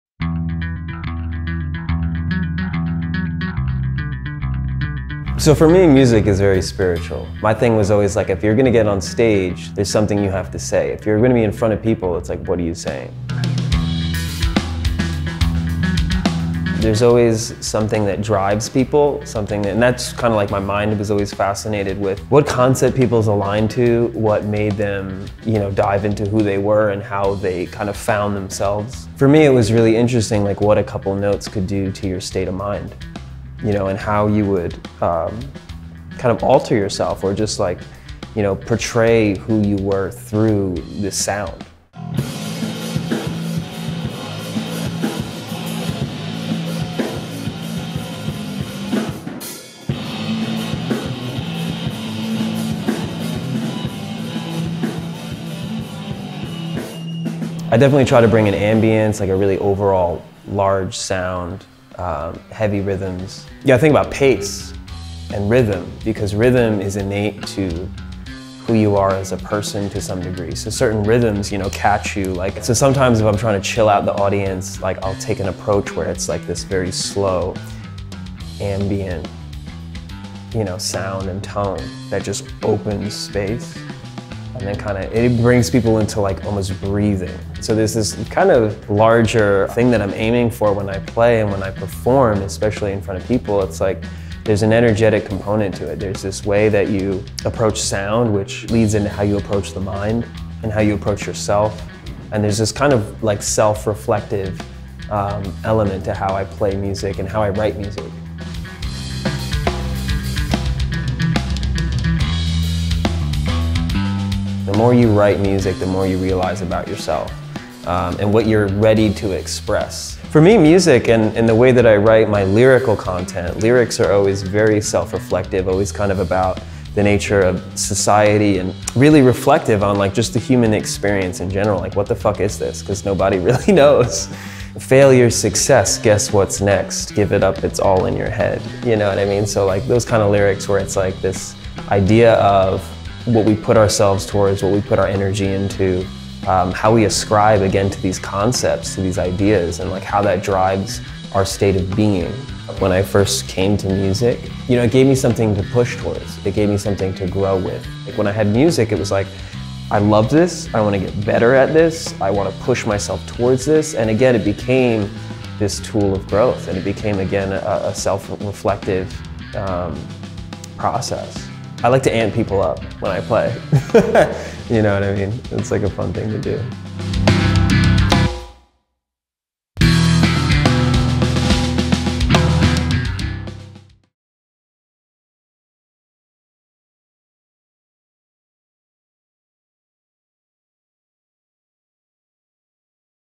Self Perpetual | Bassist, Singer/Song-writer | EMERGE Artist Profile
Self Perpetual, named Daniel Martin, is a bassist whose progressive rock influenced music seeks to explore the mind and soul.

00:00 Intro
00:16 Who You Are Through Sound
00:42 Jam
01:00 Sound & Rhythm
01:55 Writing
02:33 Importance of Music